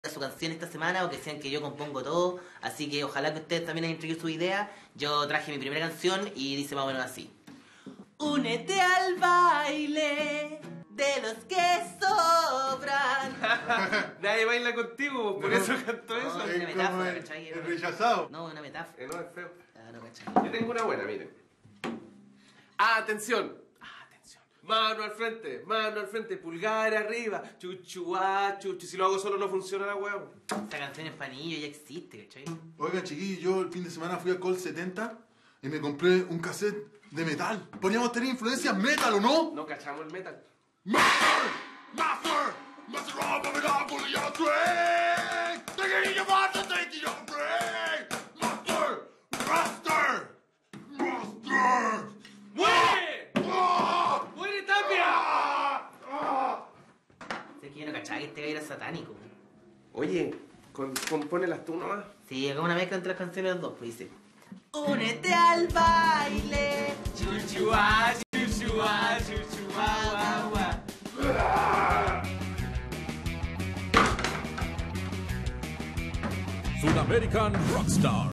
Los Sudamerican Rockstars presentan sus nuevas canciones
Ríe con la parodia a la serie sobre la banda nacional de los 80'